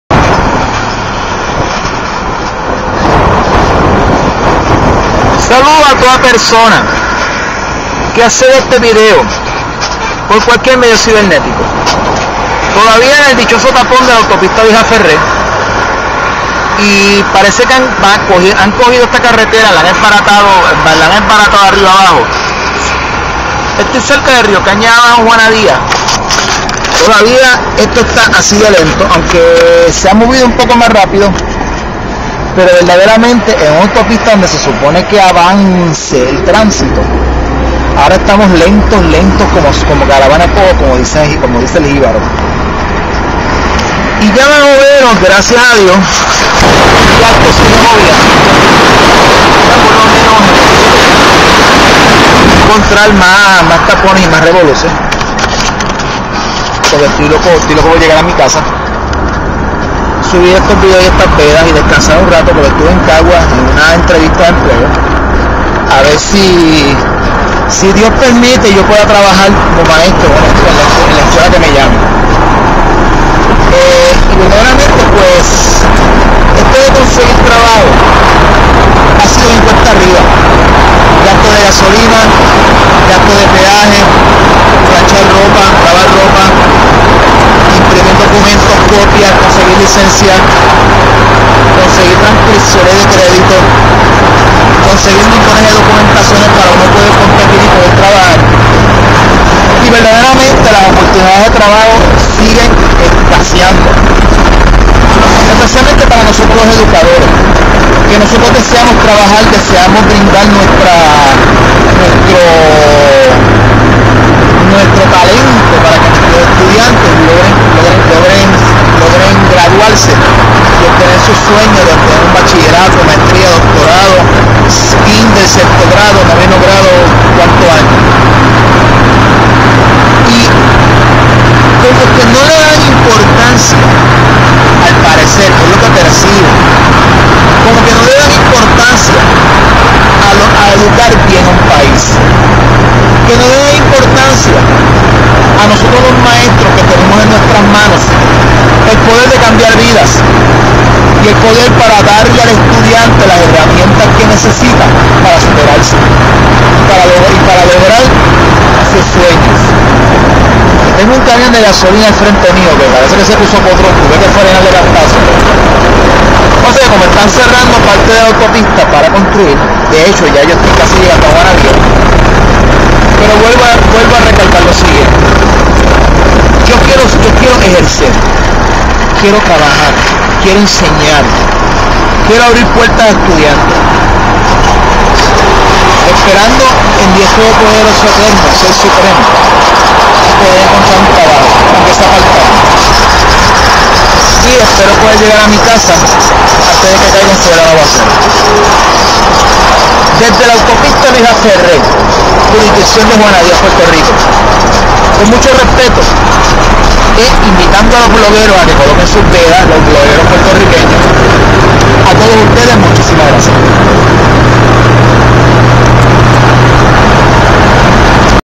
Profesor Feliciano - Veda I - Parte III - ¿Todavía estoy en el Tapón?
Halándome los pelos porque el tapón de la Autopista Luis A. Ferré no se mueve.